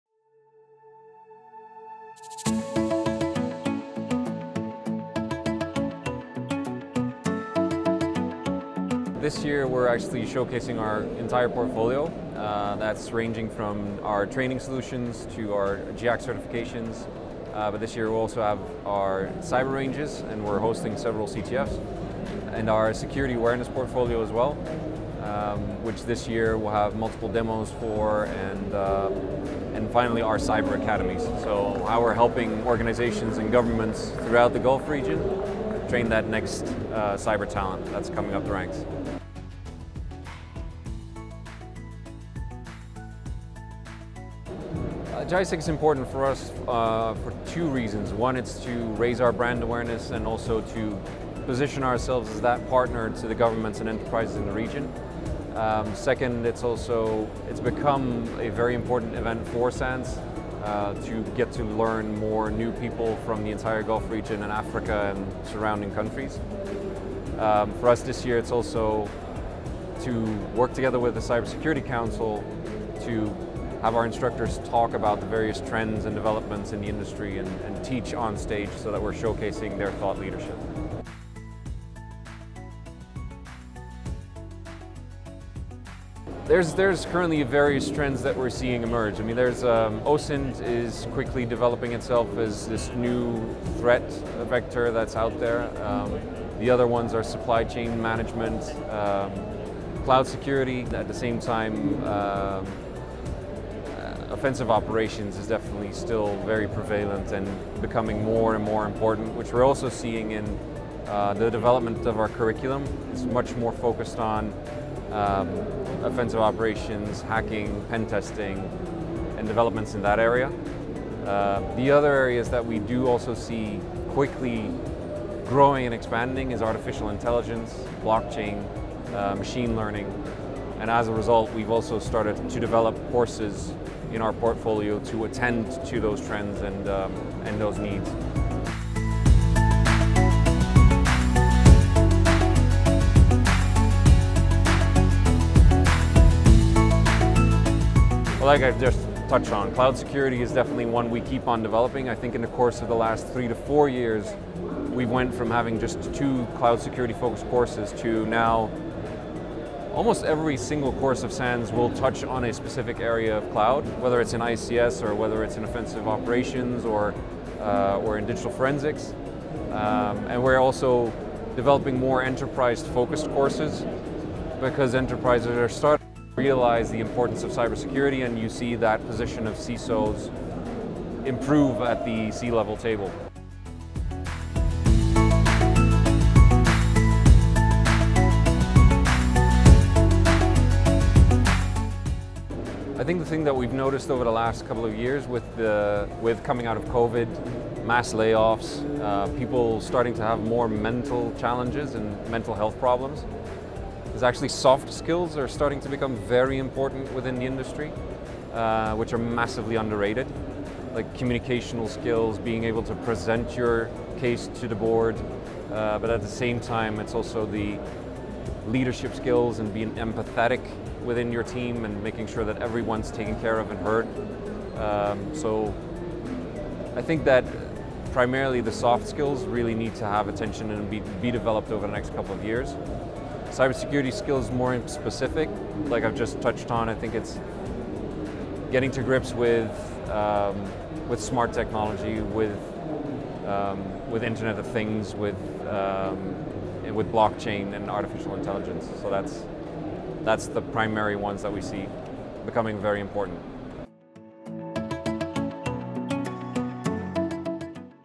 Michael Heering of SANS Institute, on why soft skills are important for cybersecurity
Cybersecurity professionals must be able to communicate and express themselves well while staying up-to-date with technology trends, says Michael Heering, Global Field Marketing at SANS Institute.